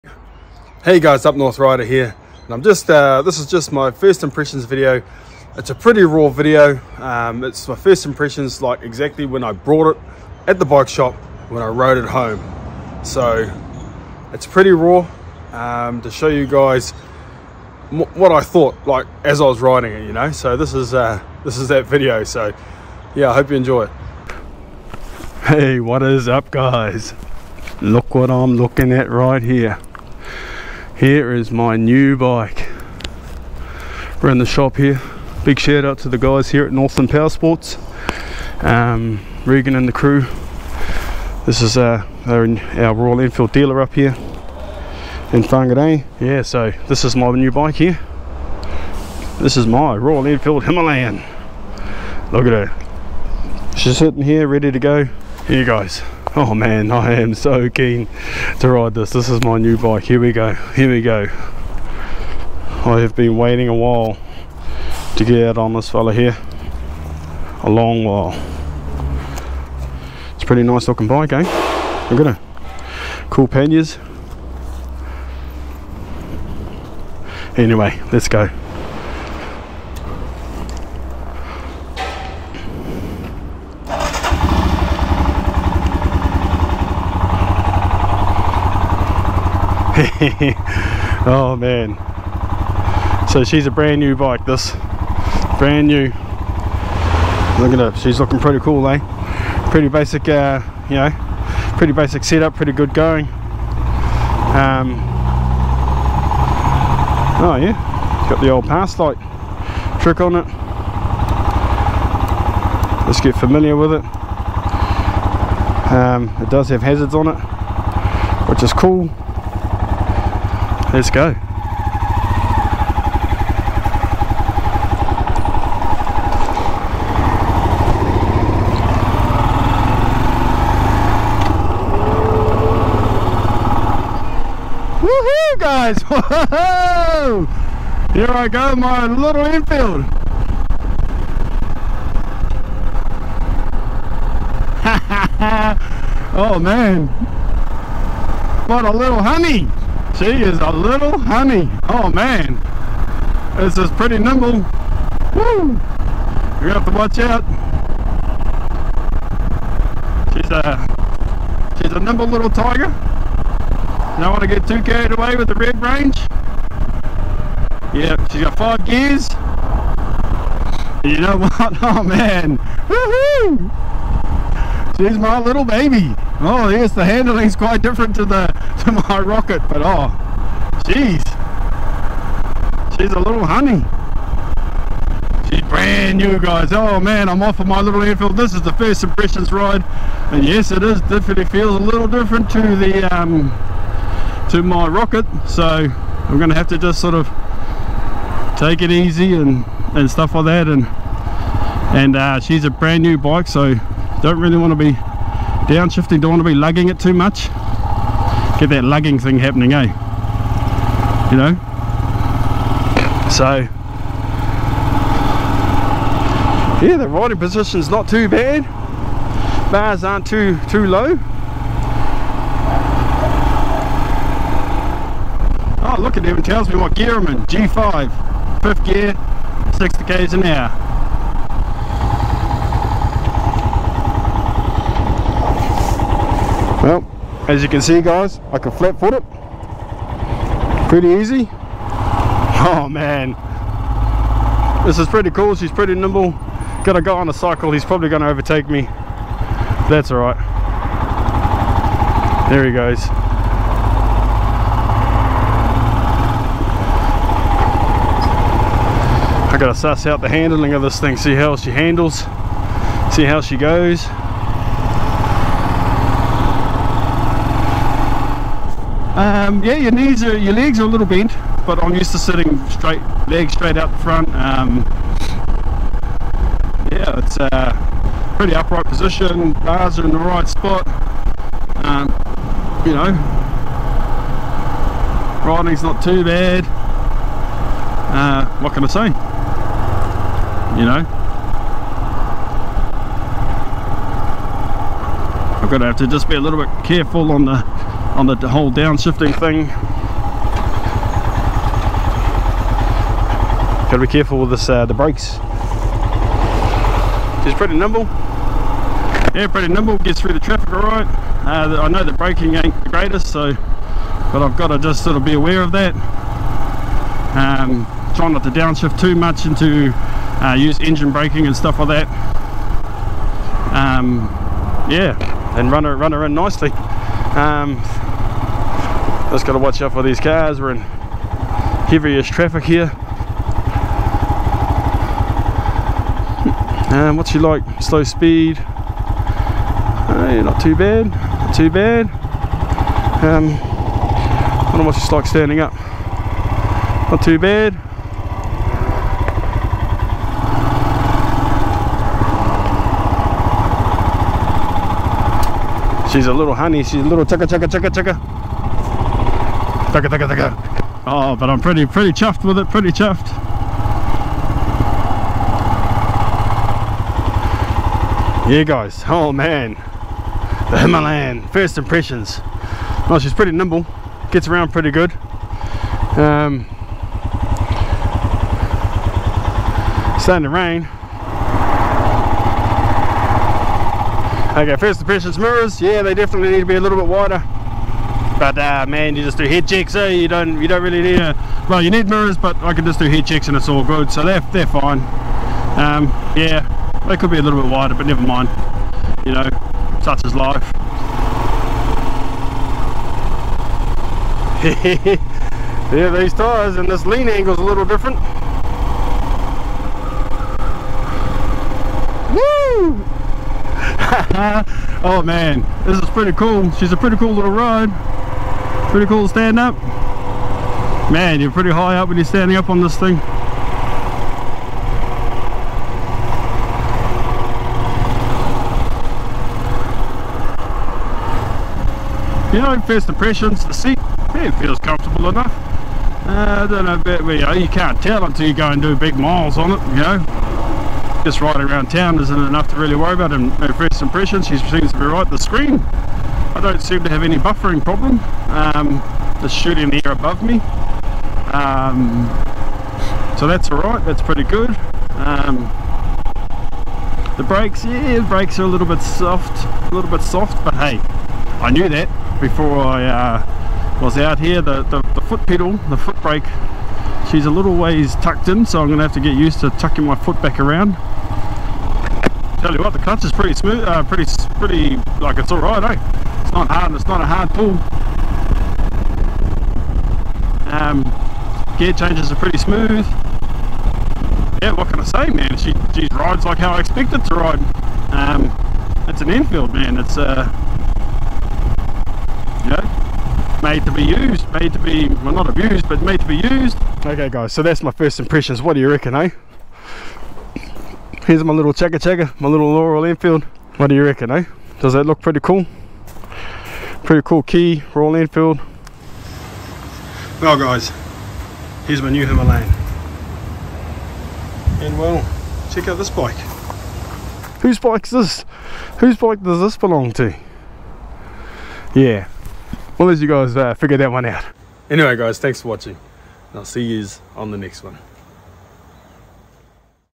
Royal Enfield Himalayan - First Impressions
Hey riders this video is a really raw, unpolished version of my first impressions of my Royal Enfield Himalayan.
This is my first ride from when I picked up the bike from Northland Powersports and then I ride it home.